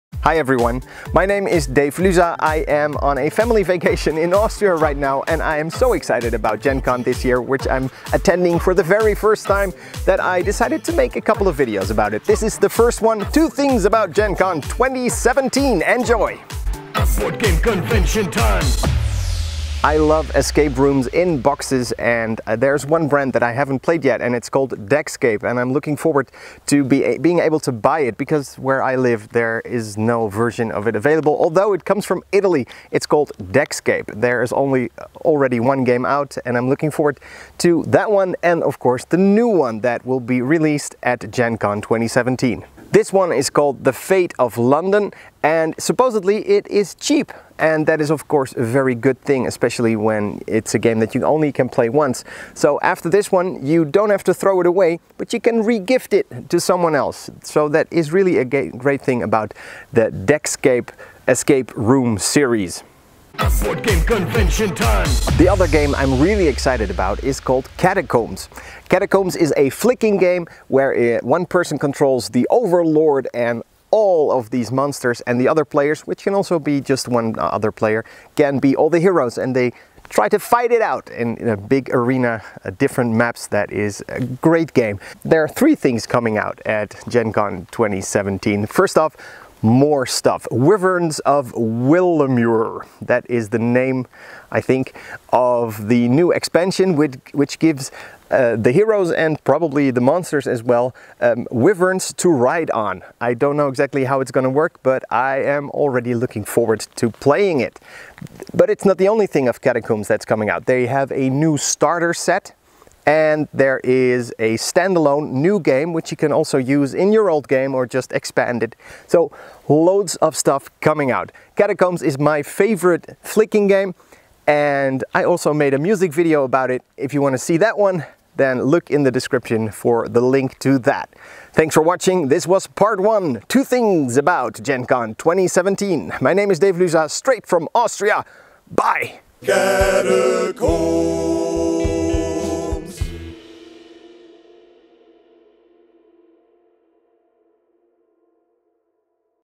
2 things about gencon volume 1
In this video I talk about two things that peek my interest at Gencon50

#1: Deckscape, fate of London
#2: Three things Catacombs

I will be posting some more of these video´s in the coming weeks.

The link to my musical look at Catacombs, as featured in  Boardgame Blender´s "Lights & Shadows" Episode.